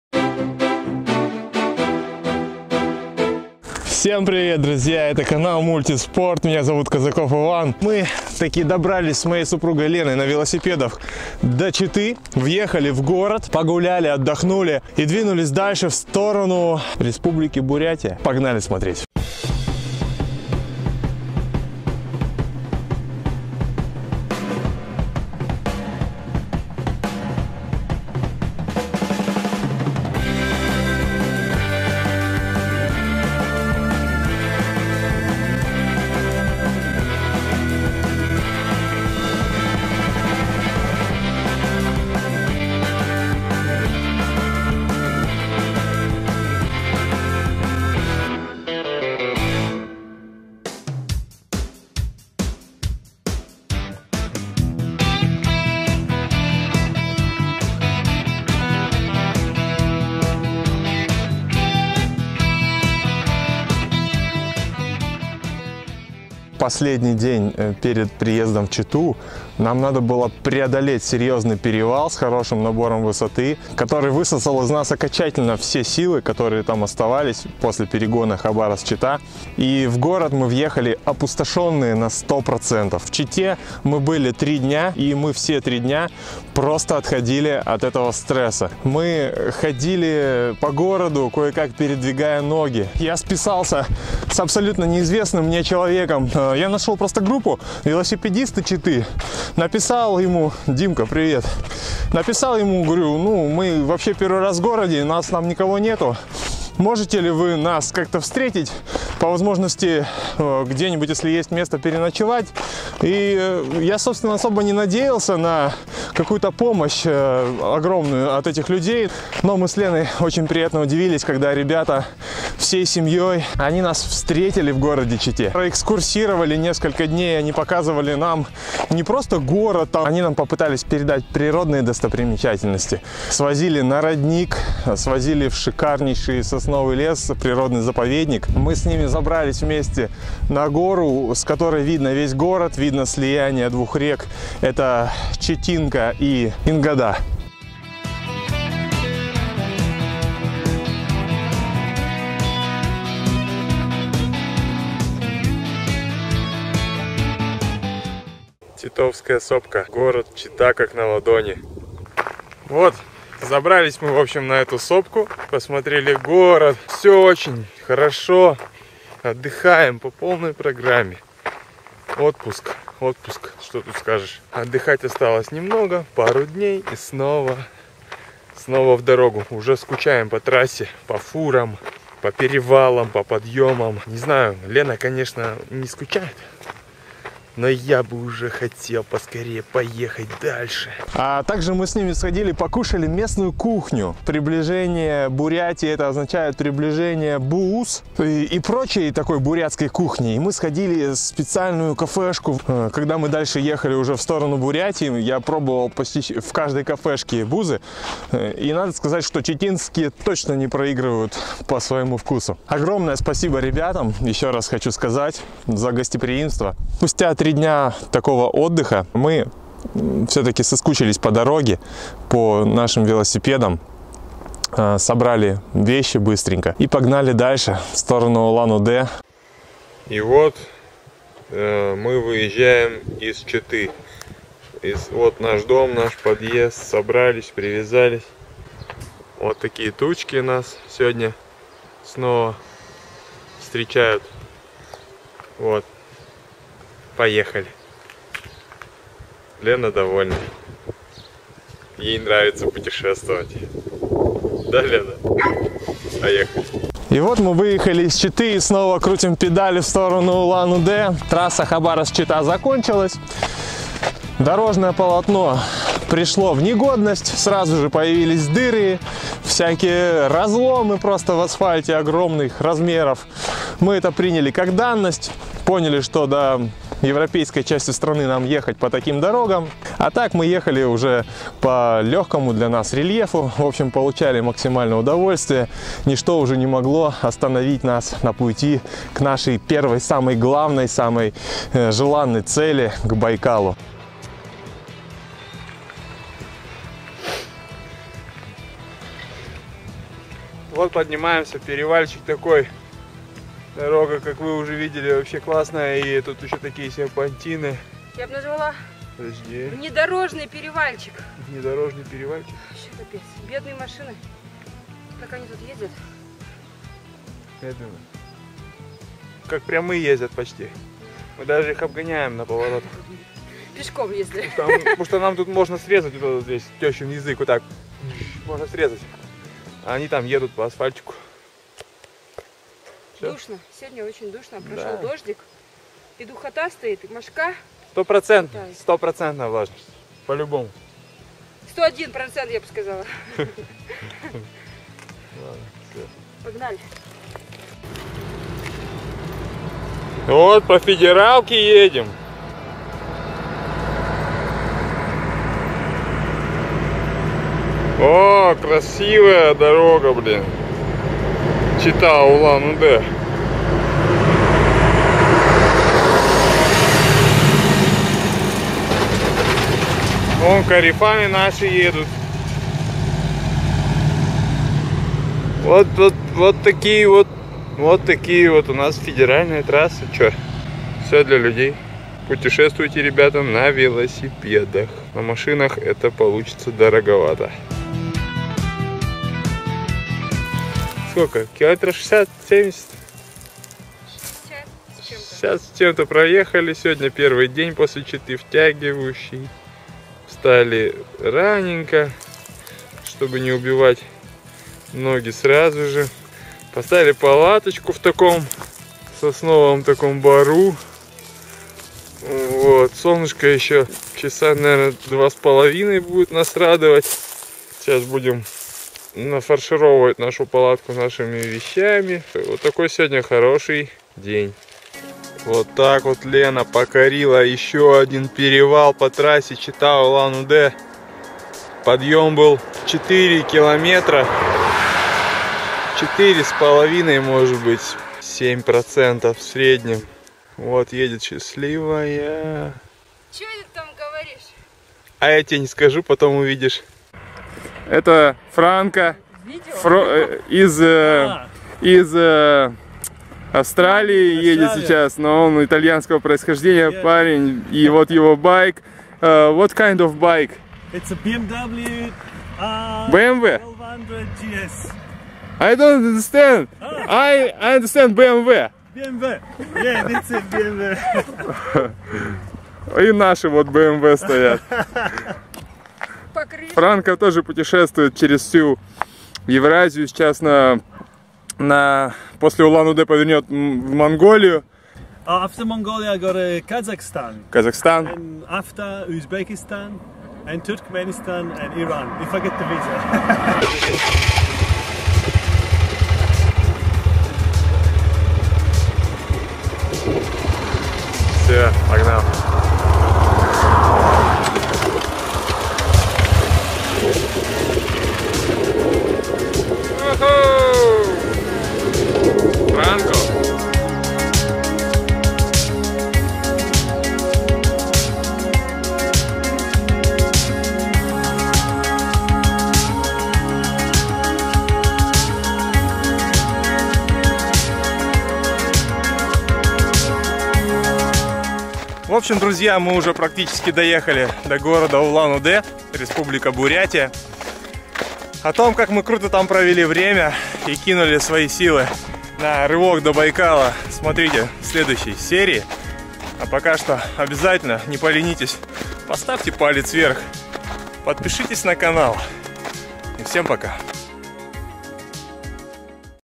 ВЕЛОТУР 10 000 км | Блог эп.5 | Чита - Улан-Удэ | Сycling across Russia ep.5 Chita Ulan-Ude
Подробный рассказ про наше семейное велопутешествие через всю страну. Хабаровск-Севастополь. 10 000 км. Хороший отдых от велосипедов в Чите, смотрим местные достопримечательности, едим буузы. Дальше дорога ведет нас к Байкалу.

Мой профиль Страва - тут есть все данные по тренировкам и активностям